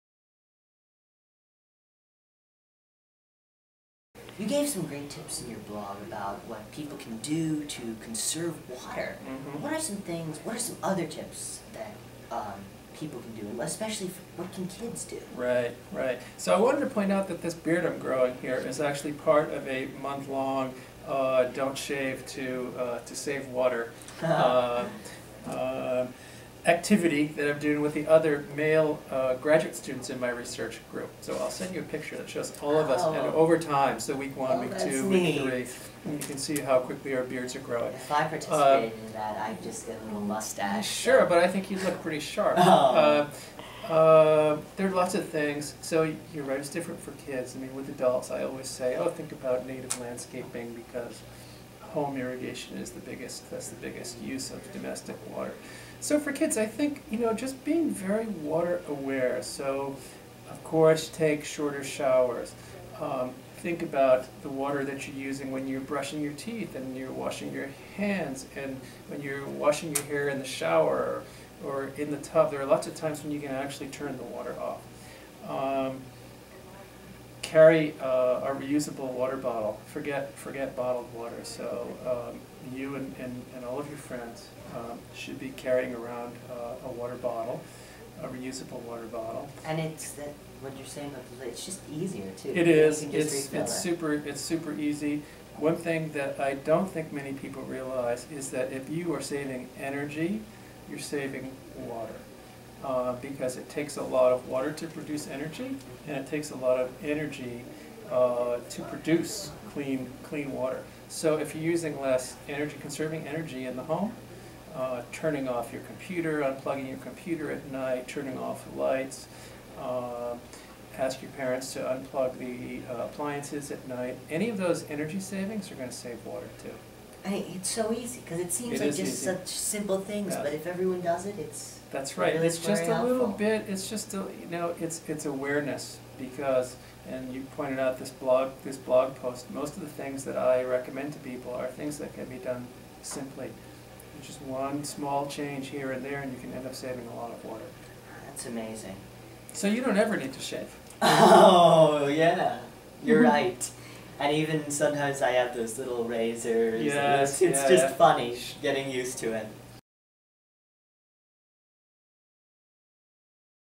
What Can Kids Do to Save Water?
Saving water begins with becoming aware of your water use. Learn about Dr. Jay Famiglietti's easy tips to help kids save water. You'll find that saving energy also helps save water too.

Dr. Famiglietti is a Professor of Hydrology with the Earth System Sciences Department at the University of California - Irvine.  His talk, "Last Call at the Oasis: Will There be Enough Water for the 21st Century?," is part of the awesome Hot Science - Cool Talks series, presented by the UT Austin Environmental Science Institute (ESI).

This is part four of Commander Ben's interview series with Dr. Famiglietti. Learn more about science and dyslexia and join the fight against invasive species